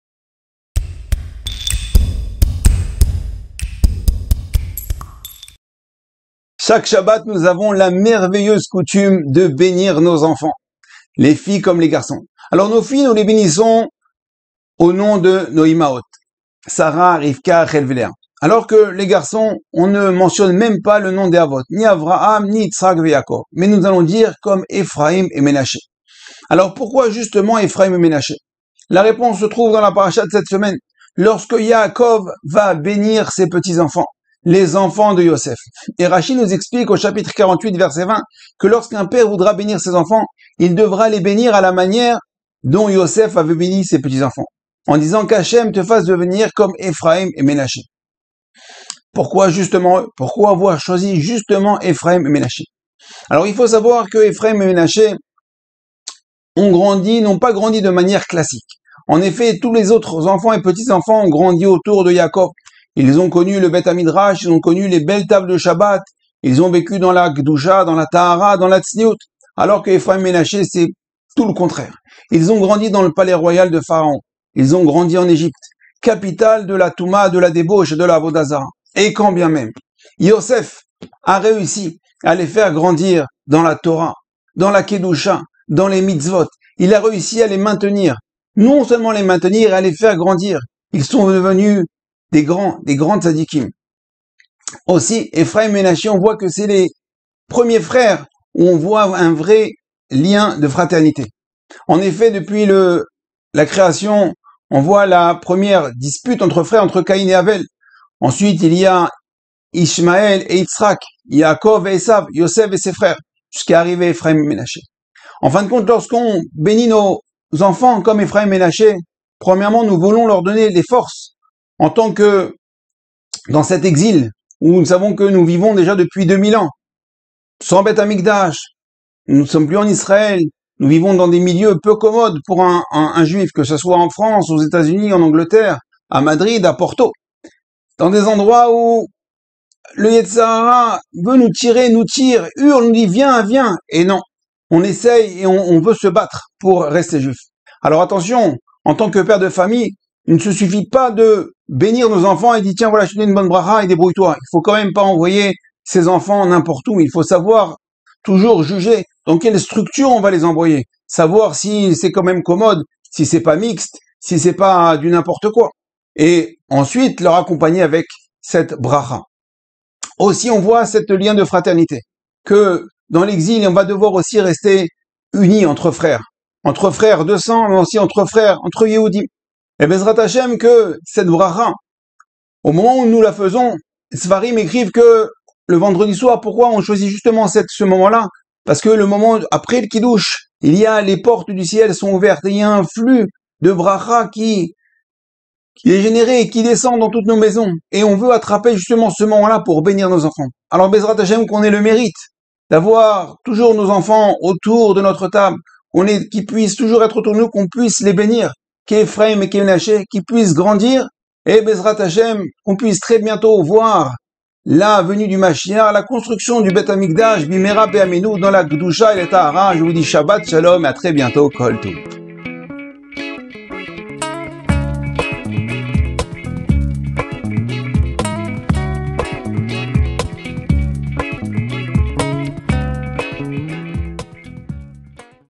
Vayehi: La bénédiction de l’exil
Nous avons la coutume que chaque parents bénissent leurs enfants le vendredi soir, les filles comme les garçons.
Les garçons seront bénie en leur souhaitant d’être « comme Ephraïm et Ménaché ». La source de cette coutume se trouve dans la paracha Vayé’hi. Ainsi est-il écrit à propos de la bénédiction prononcée par Yaakov aux deux enfants de Yossef, ses petits-fils : « Il les bénit alors et il dit : ‘ Israël te nommera dans ses bénédictions, en disant : Dieu te fasse devenir comme Éphraïm et Ménaché !’ (…) » (Béréchit 48, 20).
Rachi commente sur place : « Celui qui vient bénir ses fils, les bénira avec leur bénédiction et dira, chacun à son fils : ‘Que Dieu te fasse devenir comme Ephraïm et Ménaché’ ».
Pourquoi les deux fils de Yossef deviennent-ils le modèle des bénédictions adressées à la postérité d’Israël?